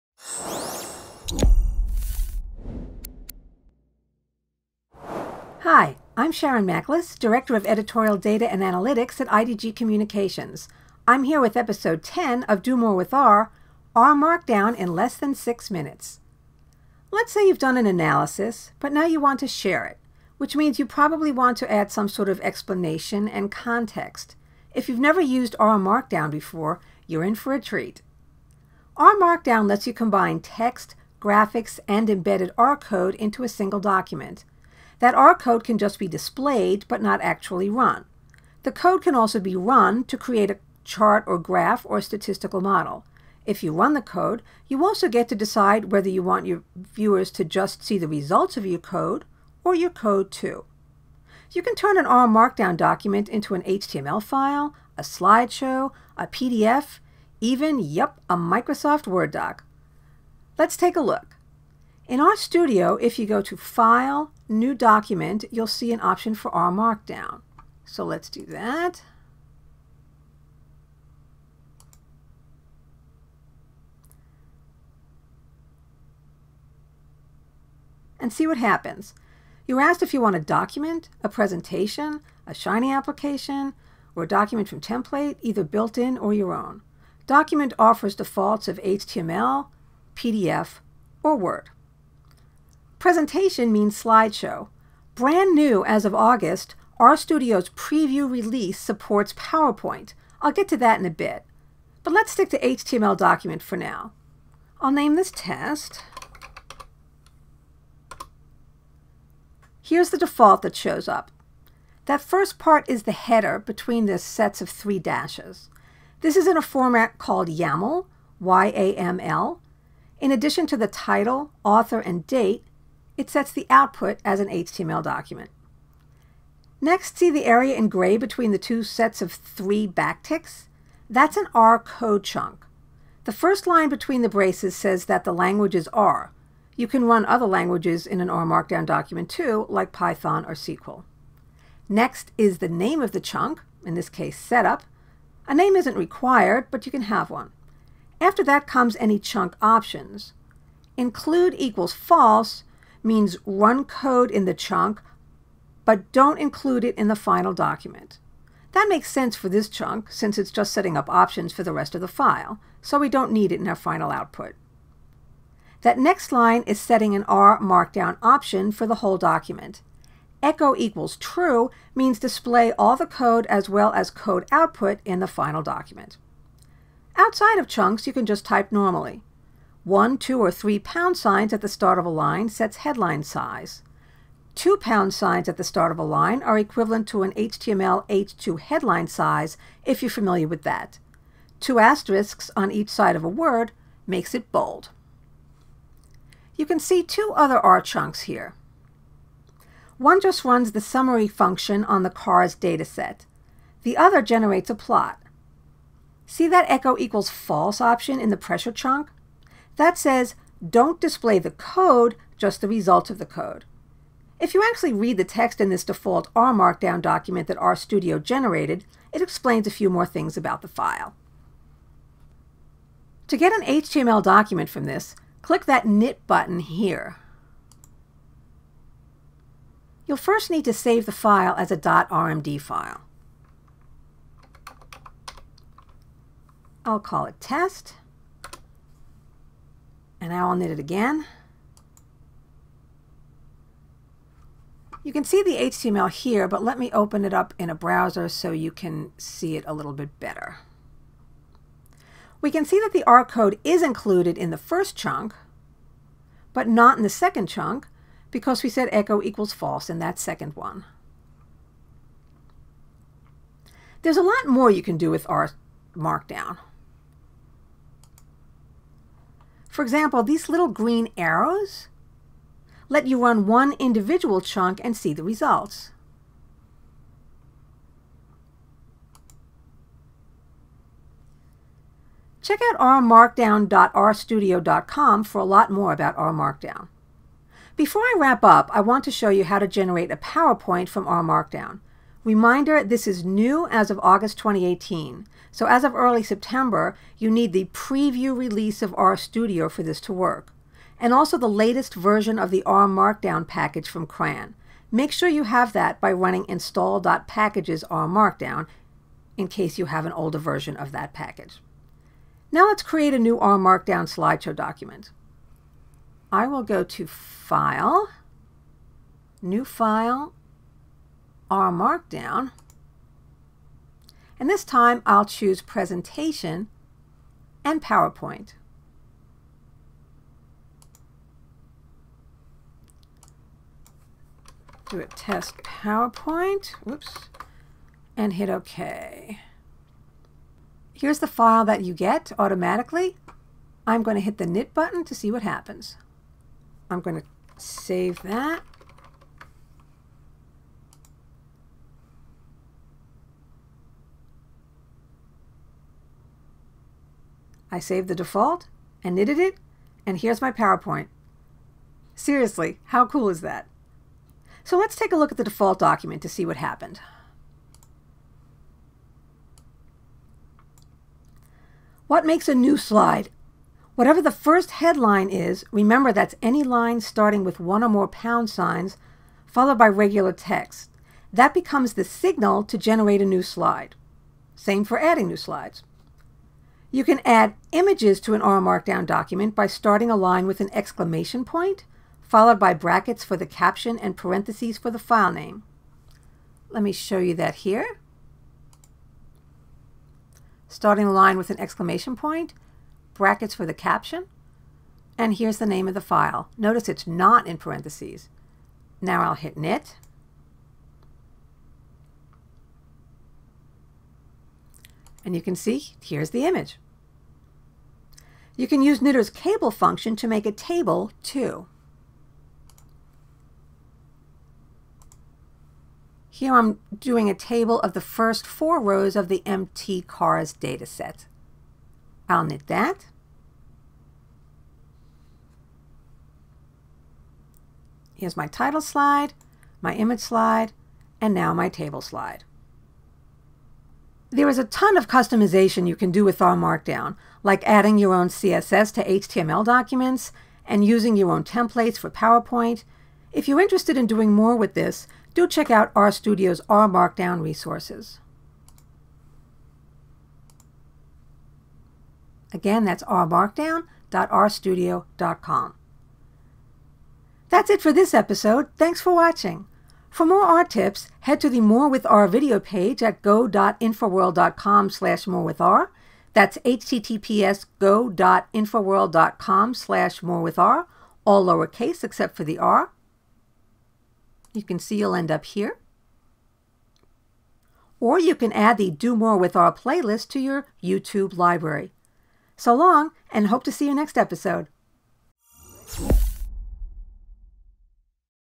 R language tip: R Markdown Tutorial
In this 10th episode of Do More with R, learn how to create HTML, Word docs, PowerPoint presentations, and more that combine text, graphics, and R code